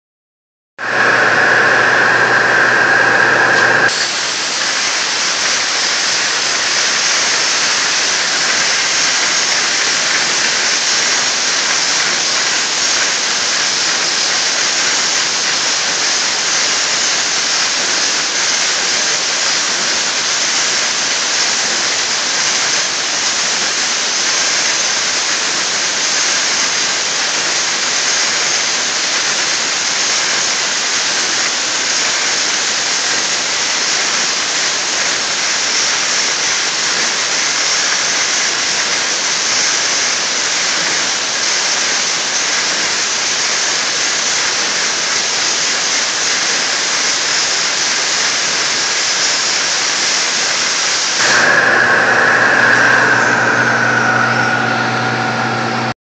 Engraving Stone
Gravando em baixo relevo com jato de água Flow.